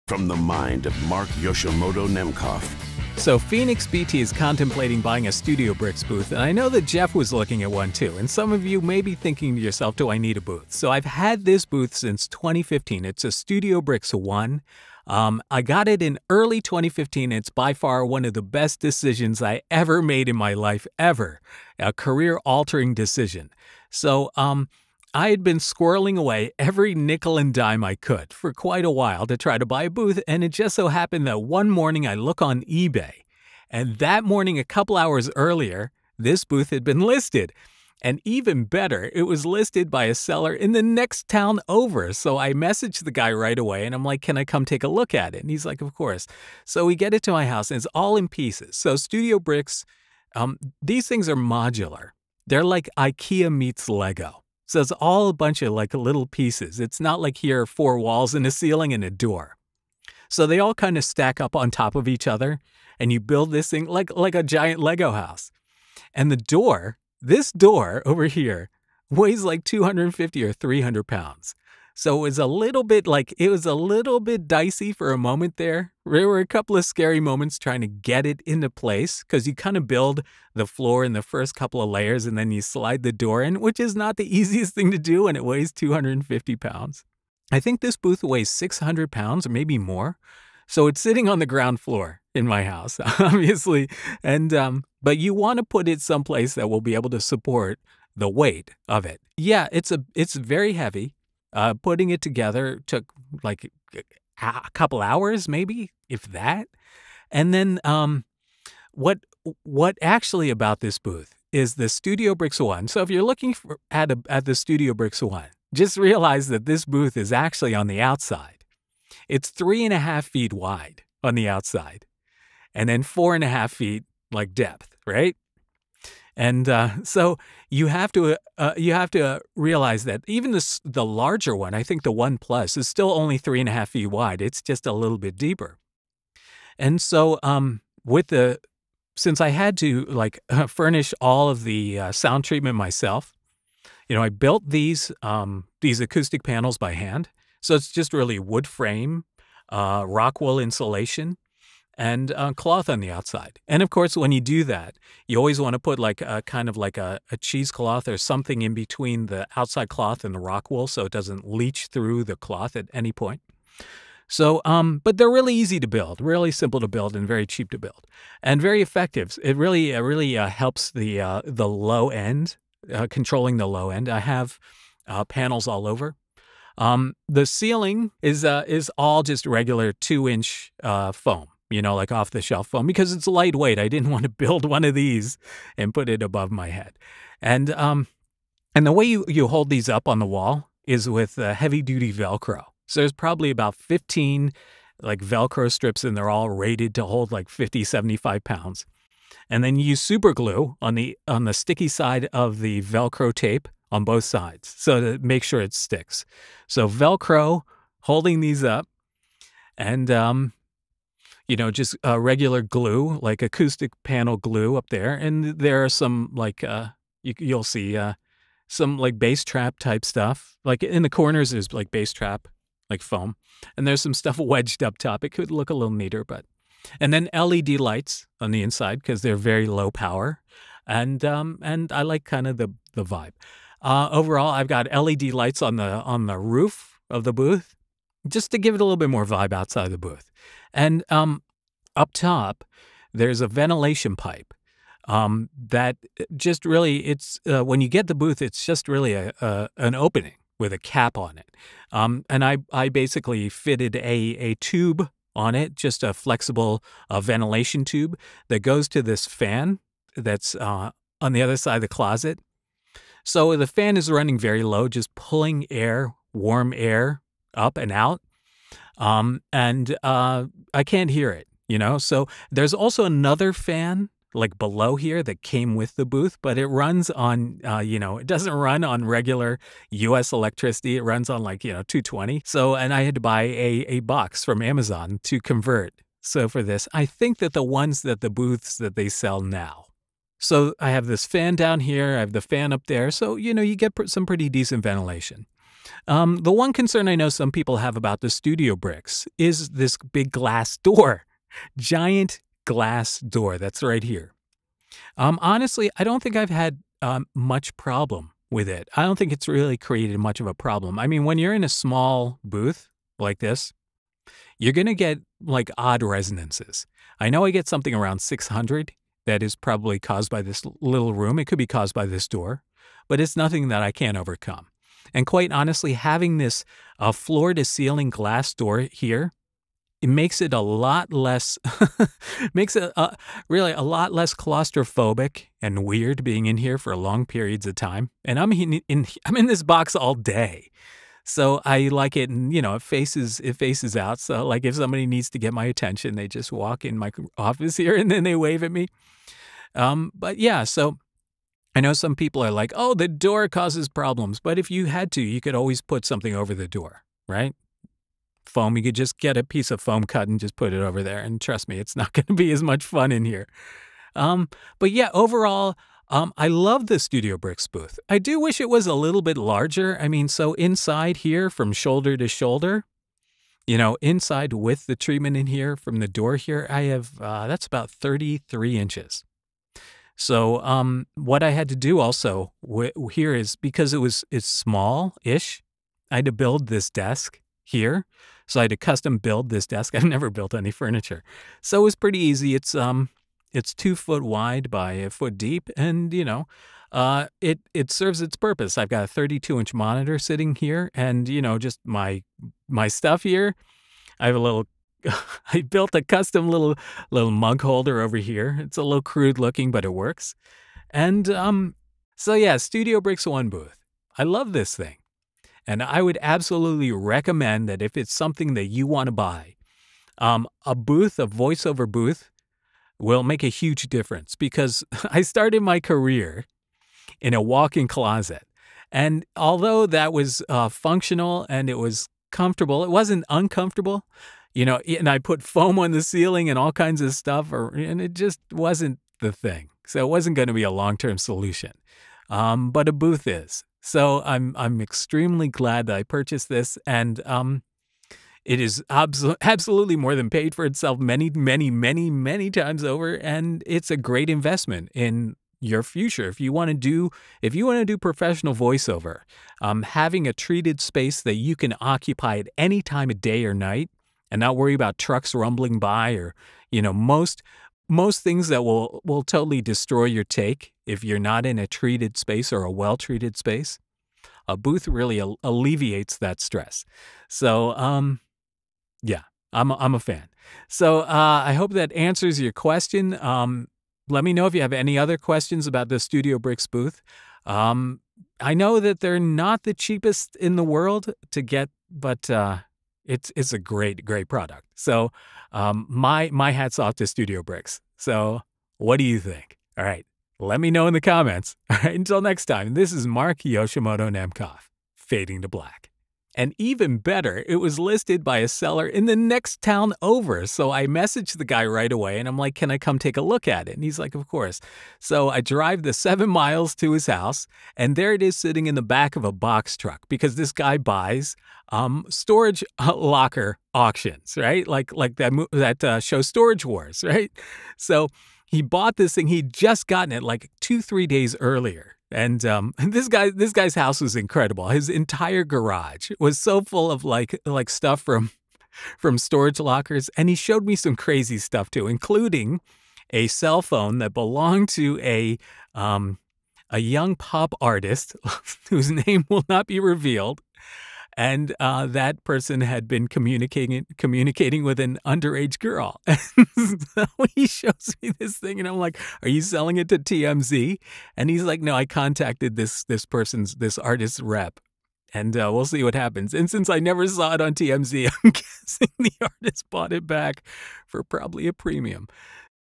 My StudioBricks One Booth for VO - A Closer Look
PhoenixBT asked for a closer look at my Studiobricks booth setup. I bought it in 2015 from a guy who buys storage locker auctions and it's been one of the best decisions I ever made. Plus, some B-roll at the end where I tell a pretty wild story about buying the booth.